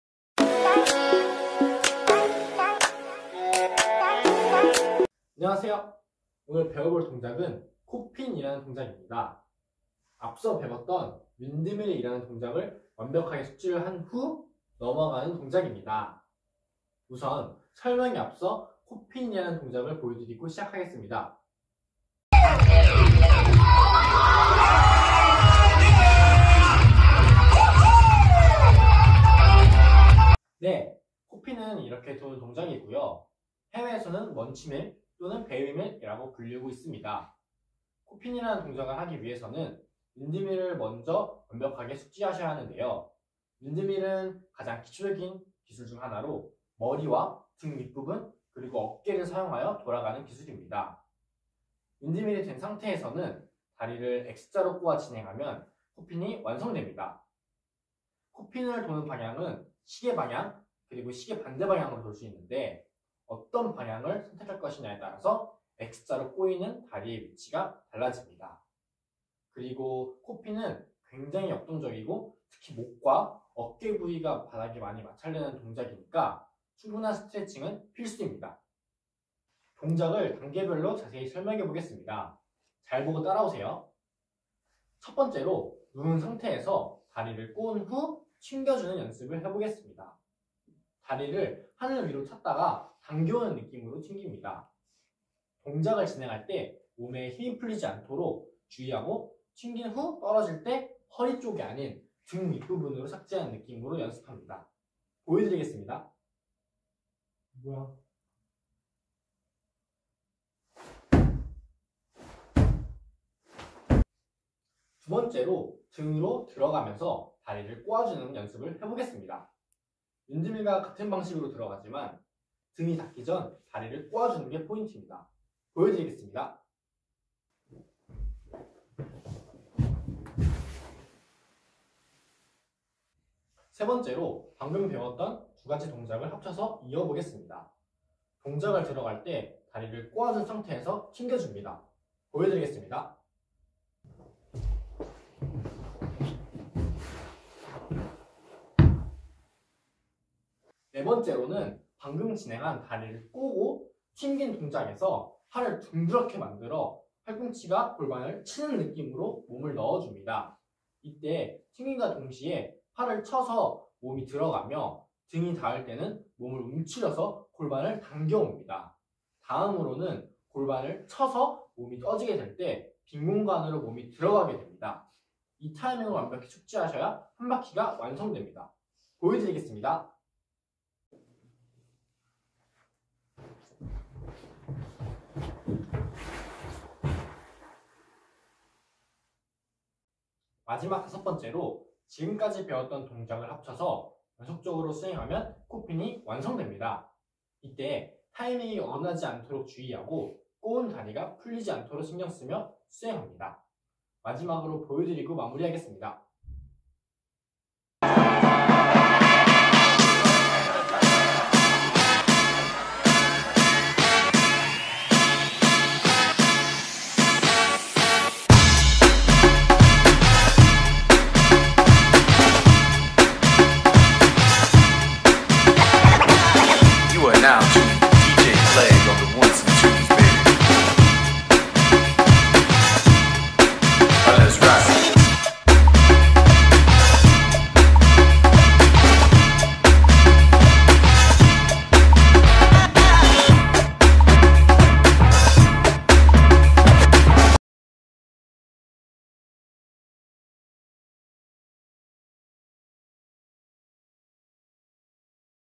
2023년 국립대학육성사업[디지털 전공 역량 개발을 위한 온라인 무브먼트 클래스] 6. 브레이킹Ⅴ(코핀)
2023 디지털 전공 역량 개발을 위한 온라인 무브먼트 클래스

제작 : 한국체육대학교 국립대학육성사업단, 공연예술학과